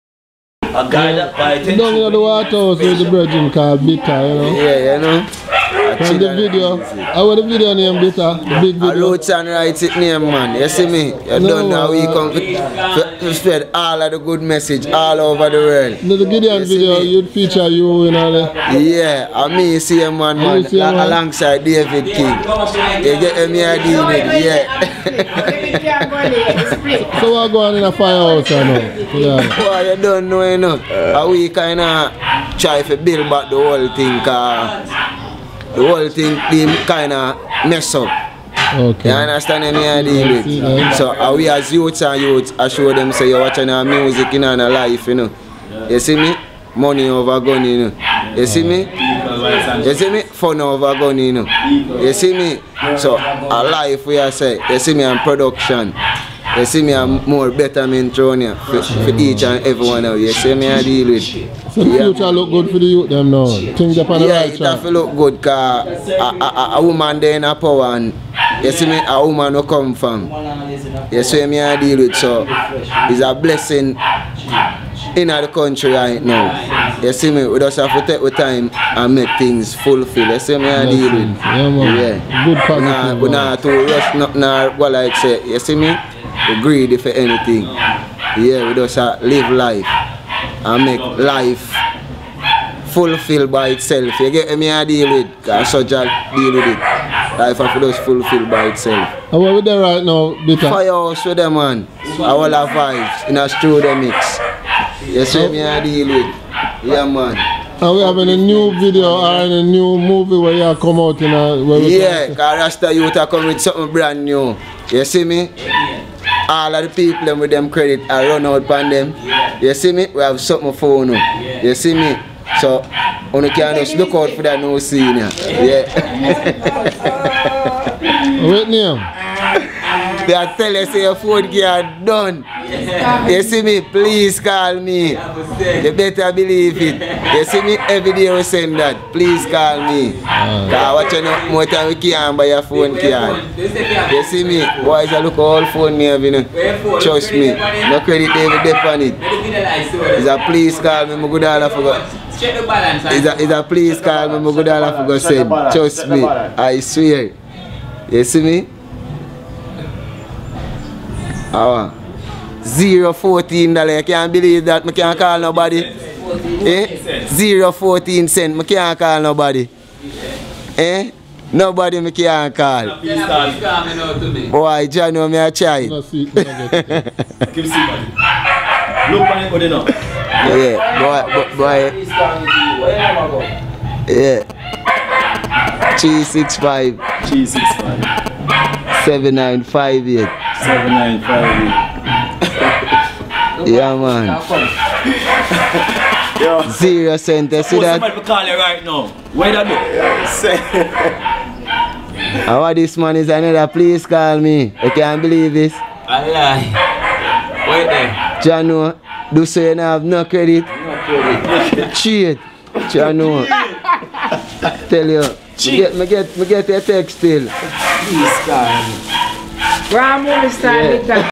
Street TV Interviews Bitta At Studio Mix Firehouse
Street TV Interviews Bitta  Blood At Studio Mix   Firehouse
A Roots & Rights Production For Fisherman Music